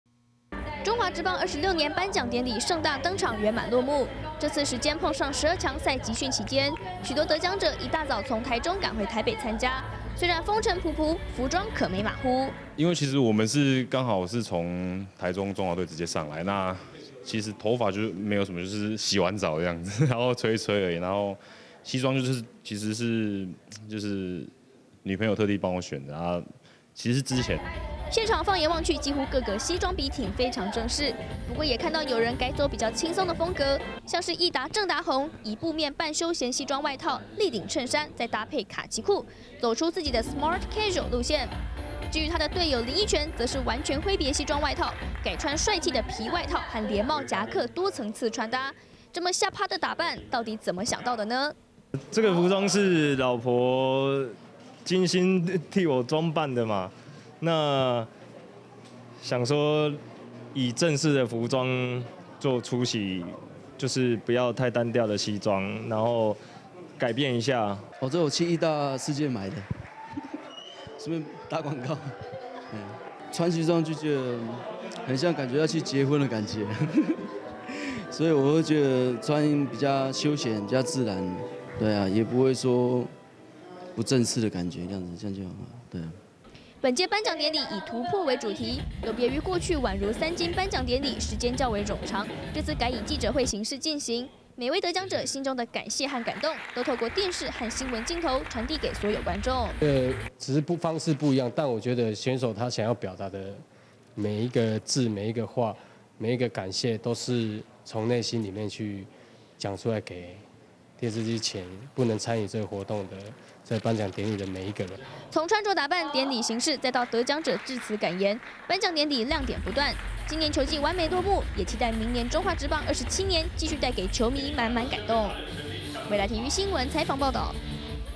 11/4 中職頒獎典禮 球員化身帥氣型男
中職頒獎典禮上選手盛裝出席，服裝也跟著成為大家討論話題。像是義大林益全穿著帥氣皮外套和連帽夾克多層次穿搭，他透漏是老婆精心挑選。另外隊友鄭達鴻也走比較休閒的打扮，他說是因為不想穿得像結婚，想要輕鬆活潑一點的氣氛。

因為碰上12強賽集訓期間，許多得獎者都是一大早才從集訓所在的台中趕來台北參加，像是林柏佑就笑說，來不及做頭髮了，幸好女朋友之前就有幫他挑一套西裝，所以趕歸趕，還是盛裝出席了。